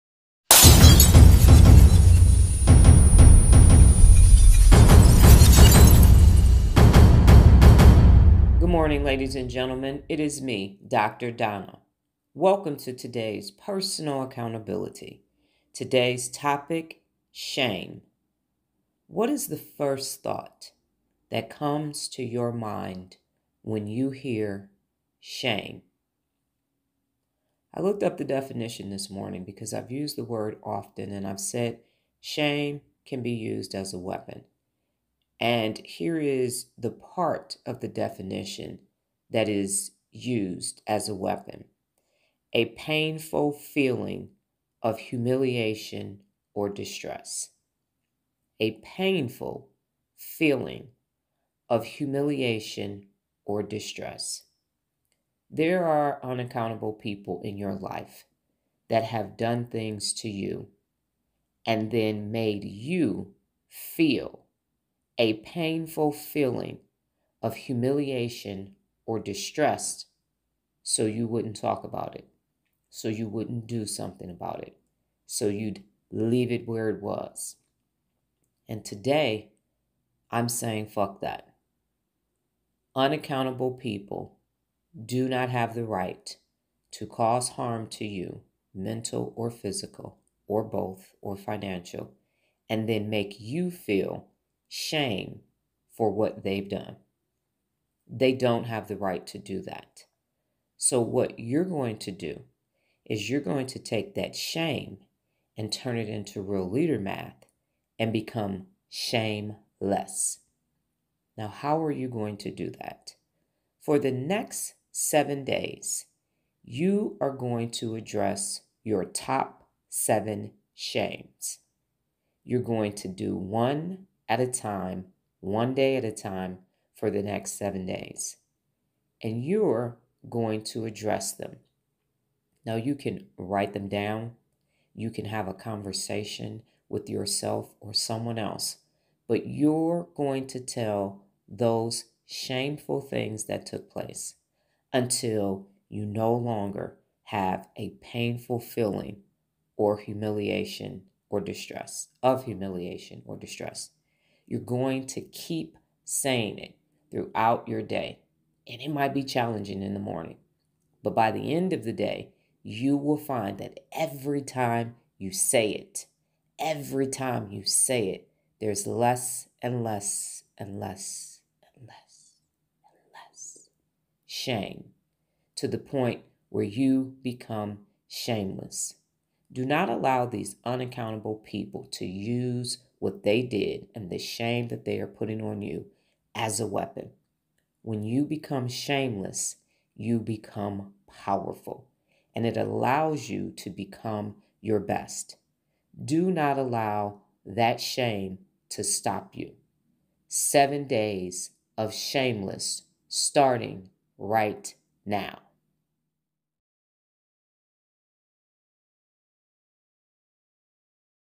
Shame ~ PersonalAccountability ~ Dr. Donna Thomas-Rodgers
Dr. Donna Talks Shame.

Listen to Dr. Donna as she provides daily personal accountability for you to assess, reflect and then take action.

Credits:

Executive Producer: Dr. Donna Thomas-Rodgers
Created by: Dr. Donna Thomas–Rodgers
Flumsitions: Power Starters Production
Production: Power Starters Production
Editing: Power Starters Production
Graphics: Dr. Donna Thomas-Rodgers
Music Produced By: Detroit Red
Genre: Red Symphony
Styling: D. Howard